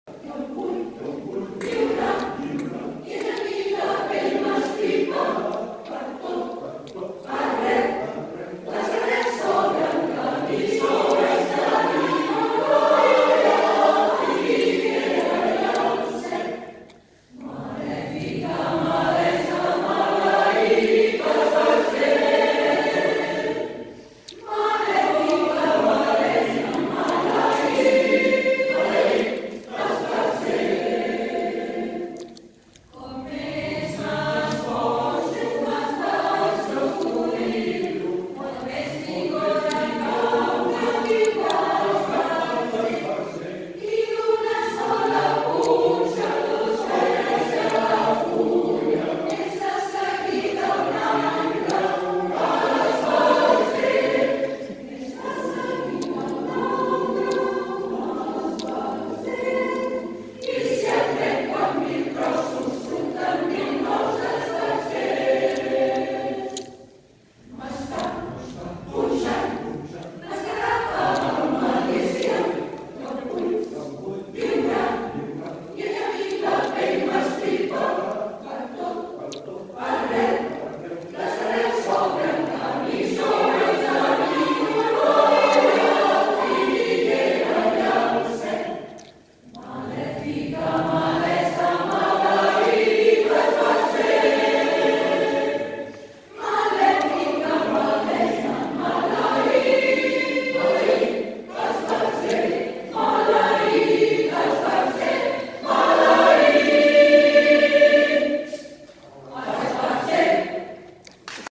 Concert a la clausura de l'Aula d'Extensió Universitaria dels EIC
Col·legi dels jesuïtes de Casp
20 de juny de 2018